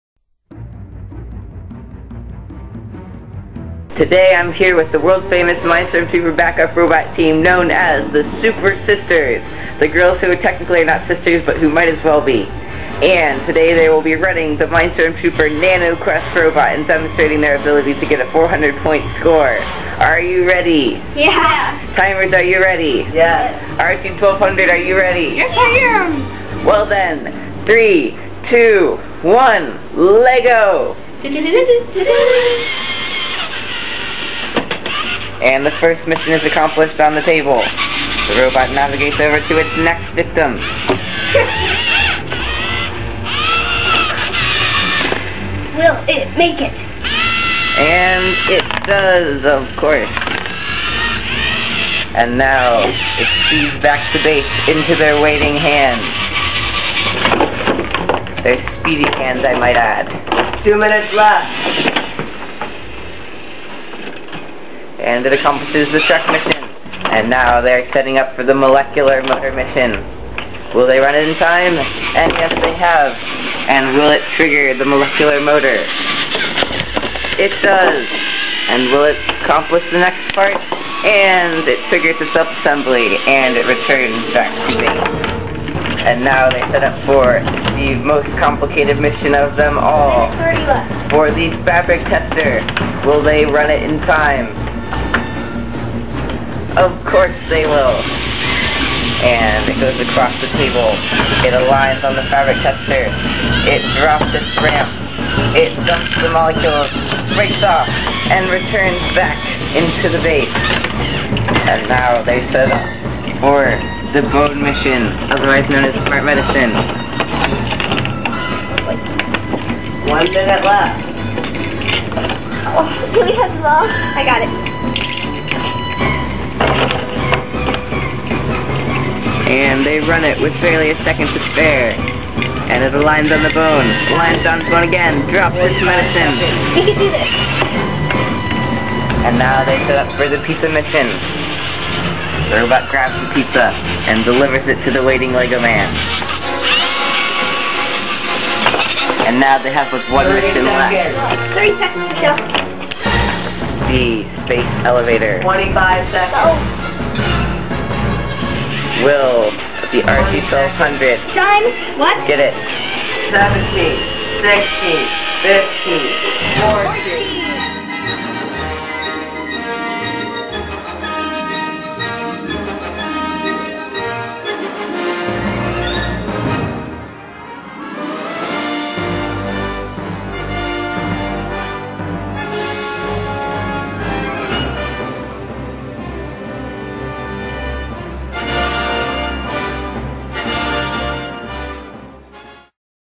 FLL 2006 - MindStorm Troopers 400 Points - Mission Demo
Demonstration of the team's 2006 robot missions.  The team later won 1st place robot performance at the FIRST Lego League World Festival for scoring three perfect rounds at the competition.